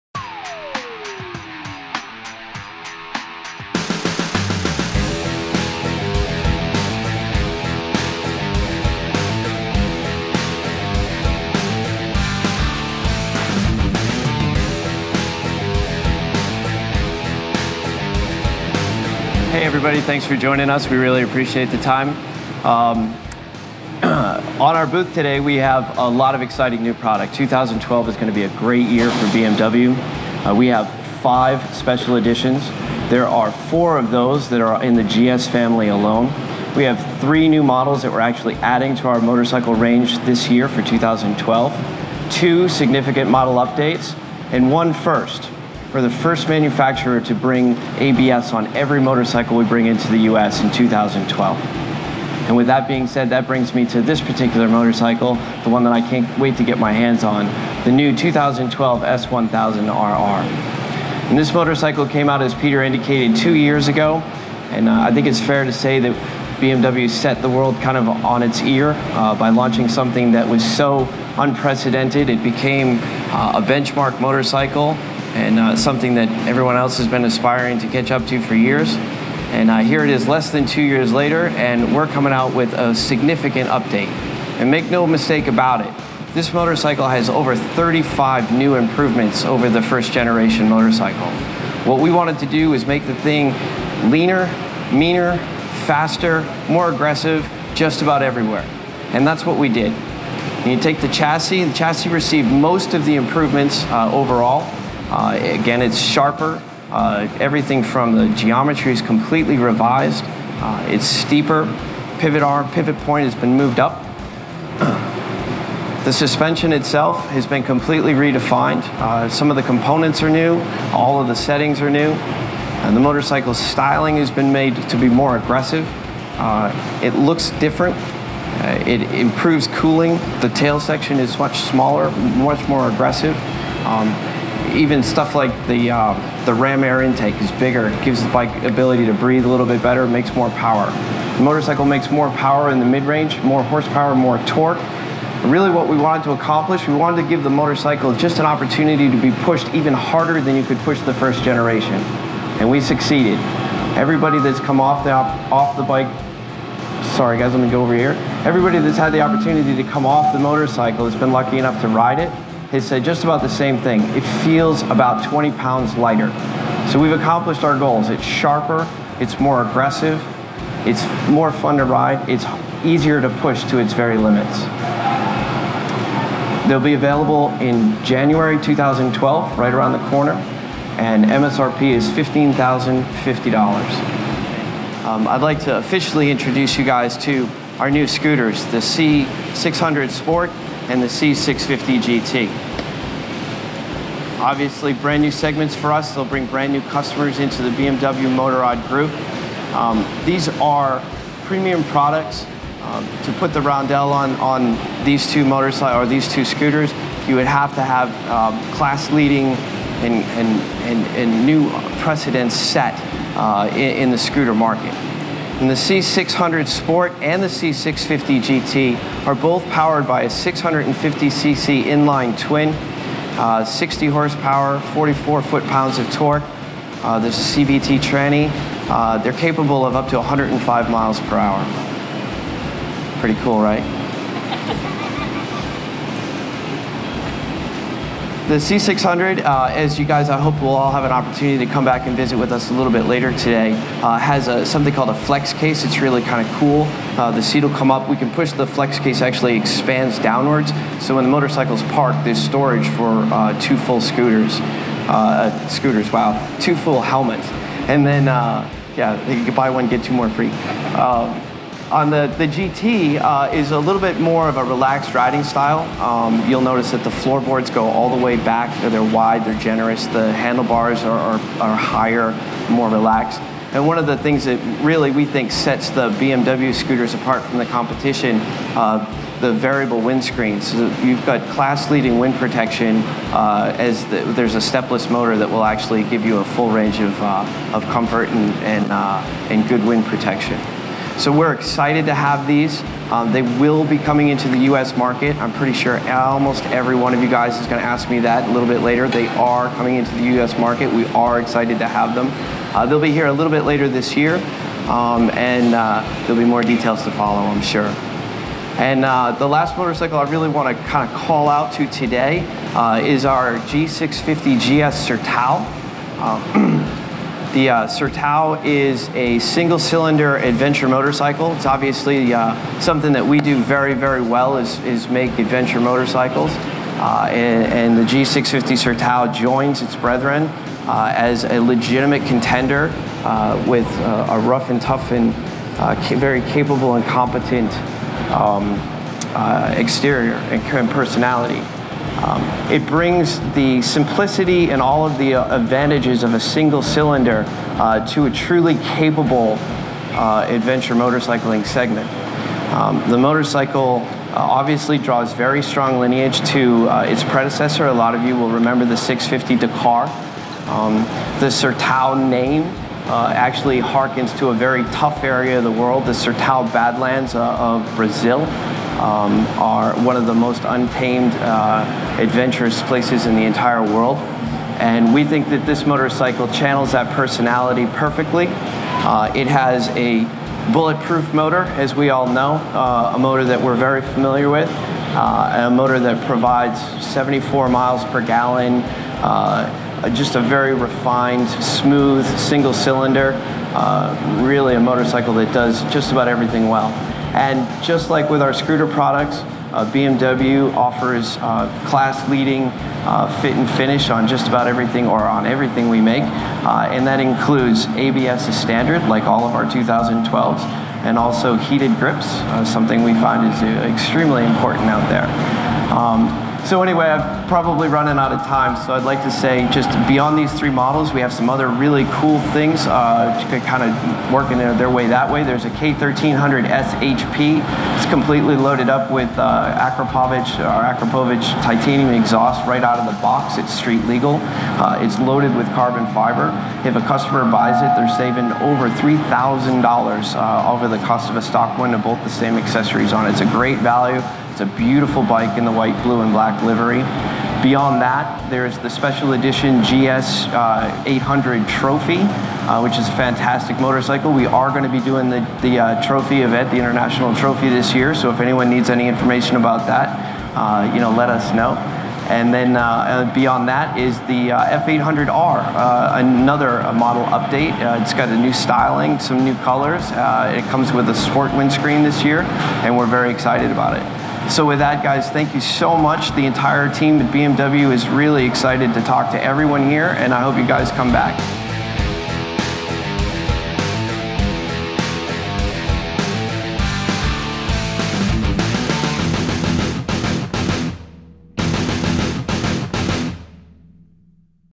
NEW 2012 BMW MOTORCYCLES at Long Beach Motorcycle Show
BMW debuted several new and revamped models of motorcycles and scooters at the Long Beach Progressive International Motorcycle show.